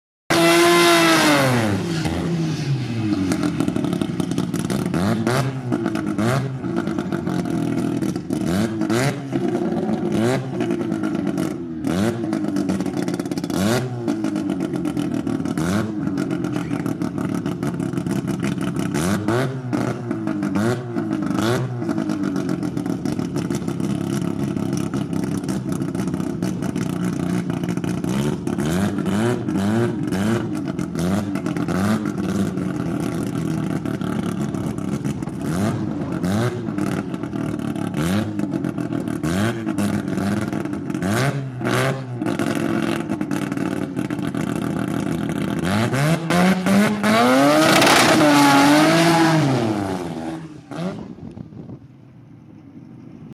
September 2020 QTM Day 1 Run 39
26th September 2020

Day 1 of this two day quarter mile event, due to racing under Covid-19 restricions no spectators were allowed and racing had to be split over two days. Today we saw practice runs, Dial in's and qualifying.

Eliminations and finals start tomorrow.